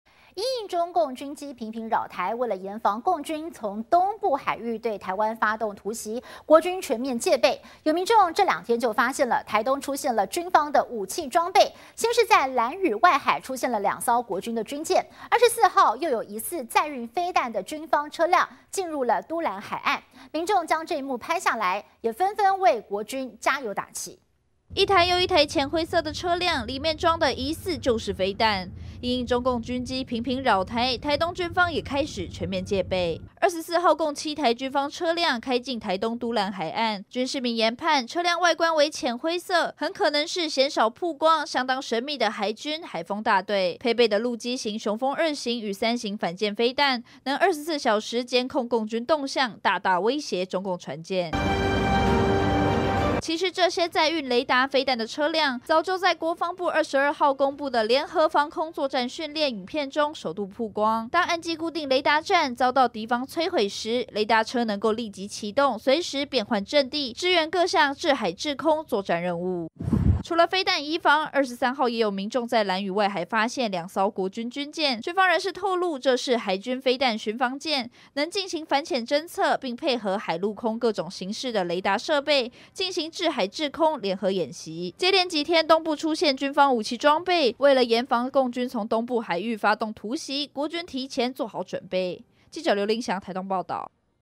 反潛偵測! 蘭嶼外海2艘"飛彈巡防艦"現蹤
因應中共軍機頻頻擾台，為了嚴防共軍從東部海域對台發動突襲，國軍全面戒備，有民眾這兩天發現，台東出現軍方武器裝備，先是在蘭嶼外海出現2艘國軍軍艦，24號又有疑似載運飛彈的軍方車輛，進入都蘭海岸，民眾將這一幕拍了下來，紛紛為國軍加油打氣。